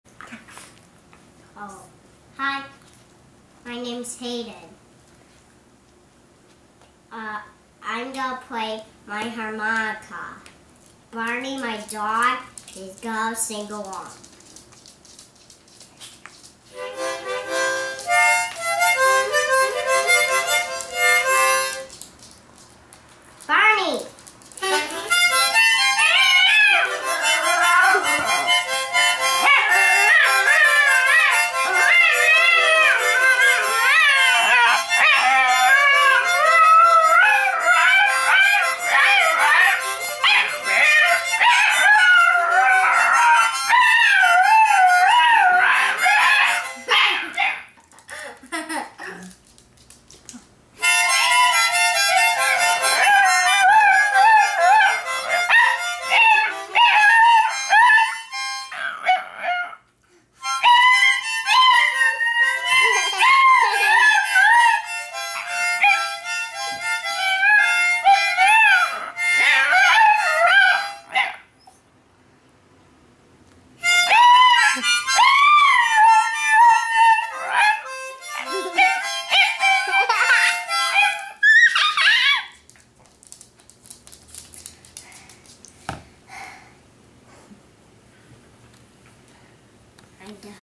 Barney sings to the harmonica!
Tune in to view and hear one of the finest  virtuoso performances of a canine singing along with the harmonica play of a young lad.